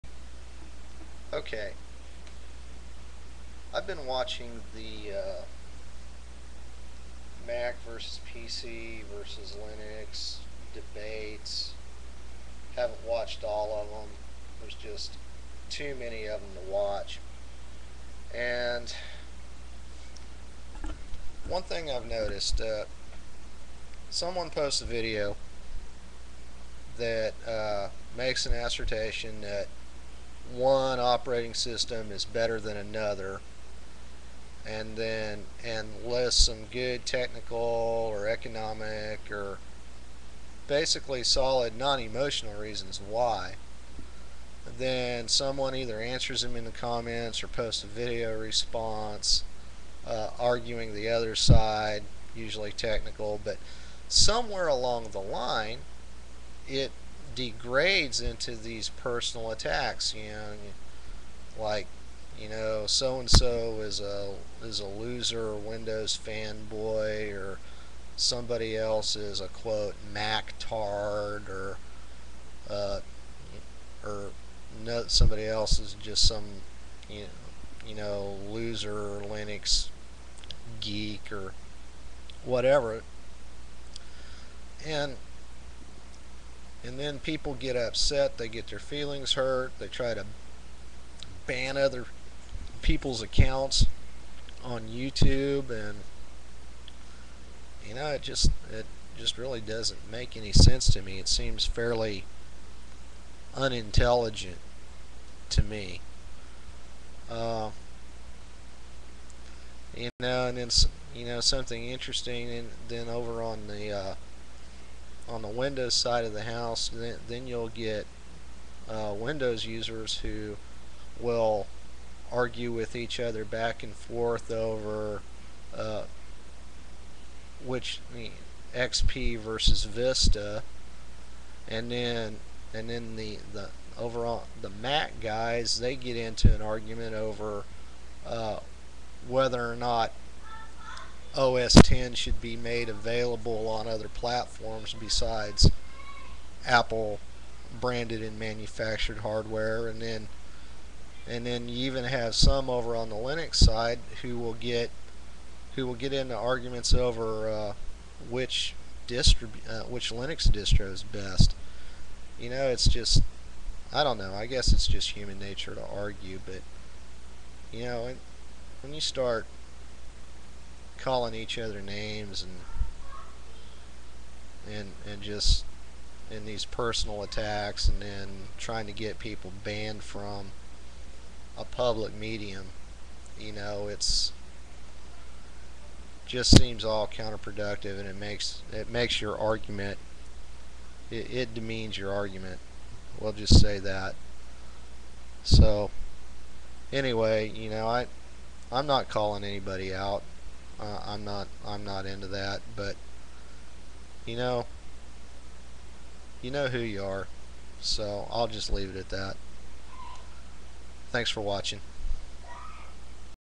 Why?
Why do technical arguments devolve into personal attacks?